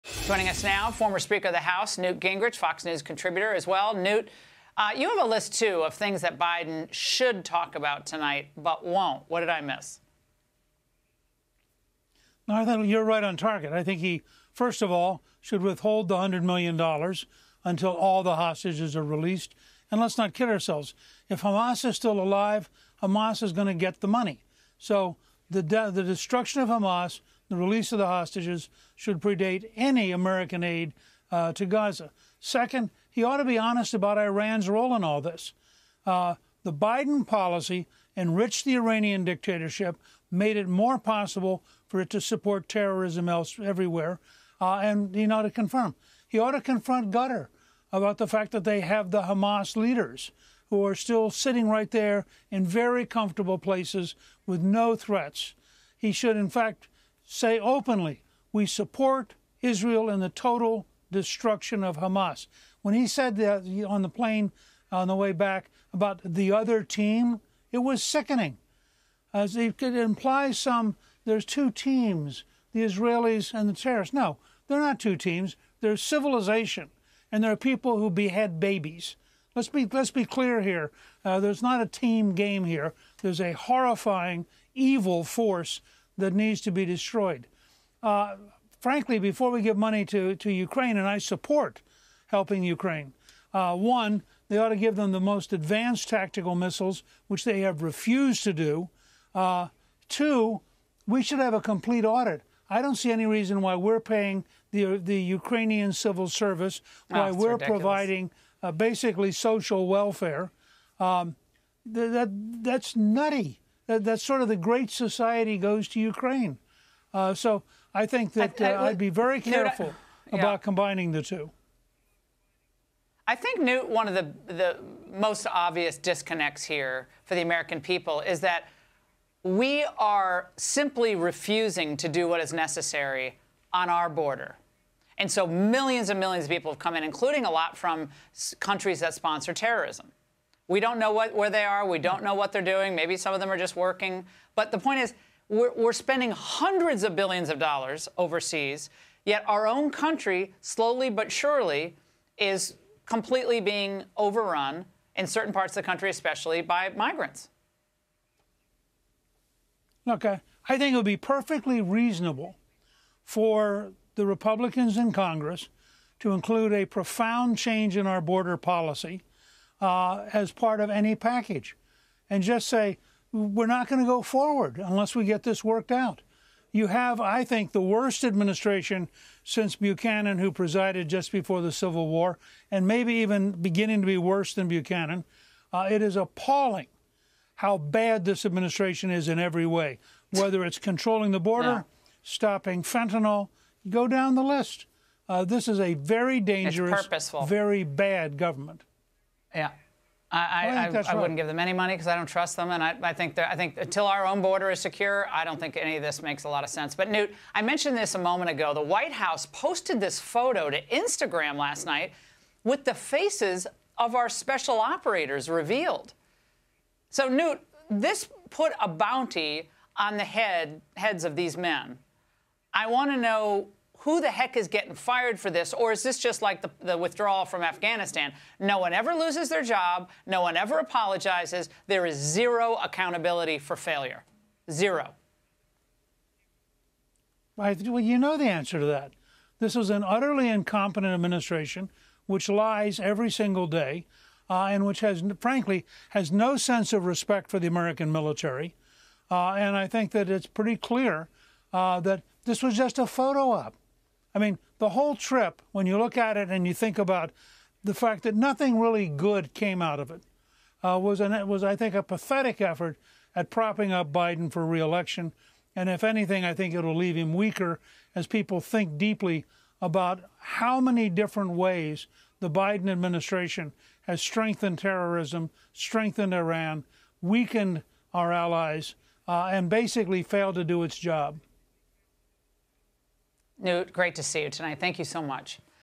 Newt Gingrich: This administration is utterly incompetent
Former House Speaker Newt Gingrich discusses President Biden's trip to Israel.